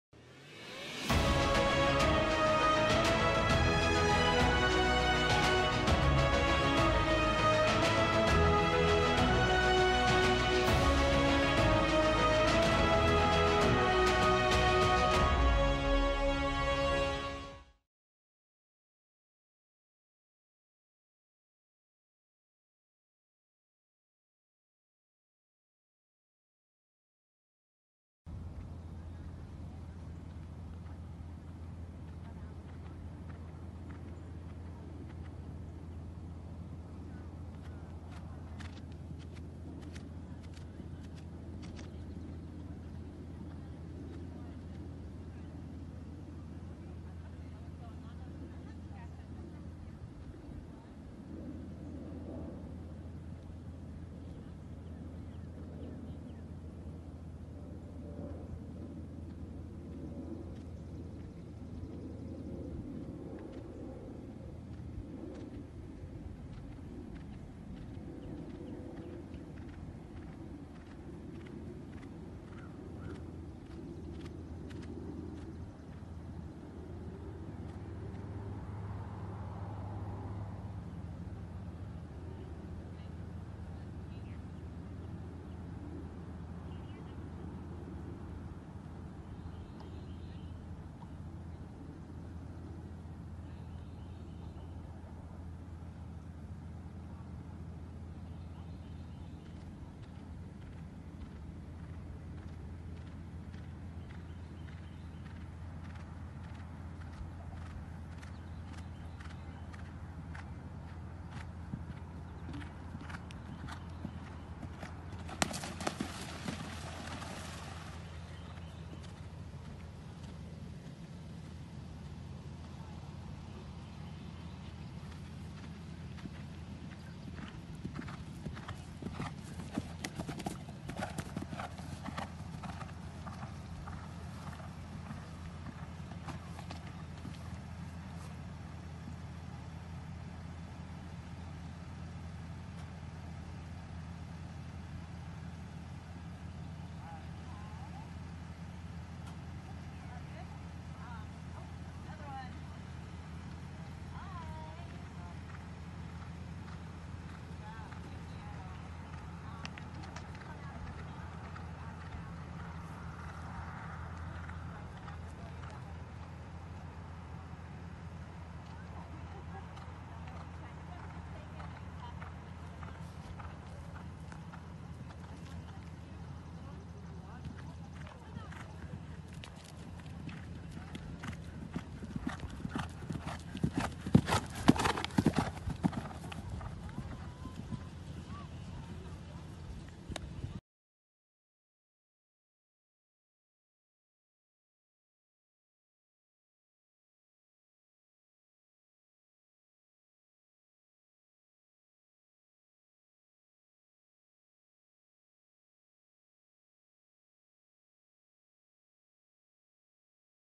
BSF Stratford ** New Event Horse Cross Country MDHT
BSF Stratford
2014 | Gelding | 16.1h | Swedish Warmblood
Shakespeare RSF x Sahara

Owned and bred by Beall Spring Farm, full brother to Frame Seahawk and Frame Shamrock.

New Event Horse | The Maryland Horse Trials II at Loch Moy